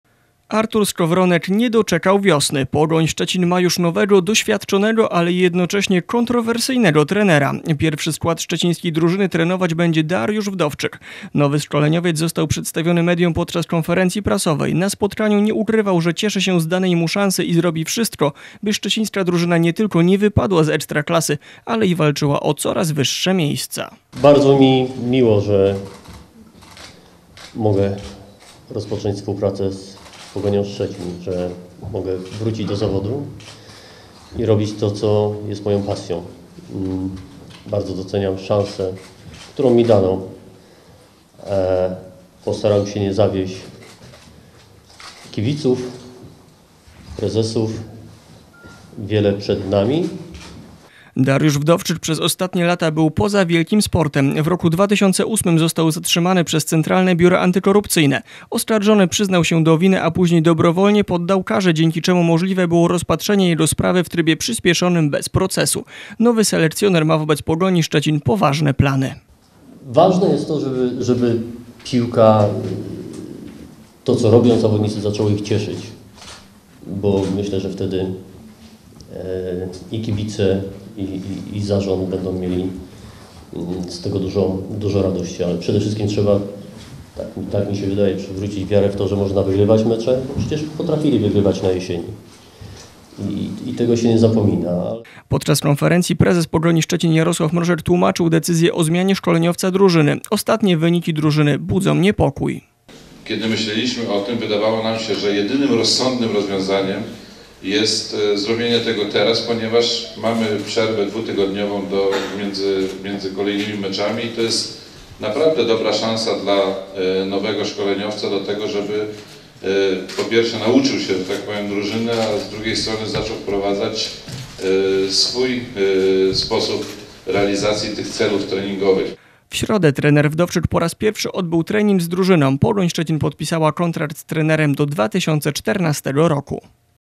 Dariusz Wdowczyk trenerem Pogoni Szczecin
Zamiast Artura Skowronka Dariusz Wdowczyk. Pogoń Szczecin kolejny raz zmienia trenera pierwszego składu zespołu. Powód jest prosty - coraz gorsze wyniki szczecińskiej drużyny.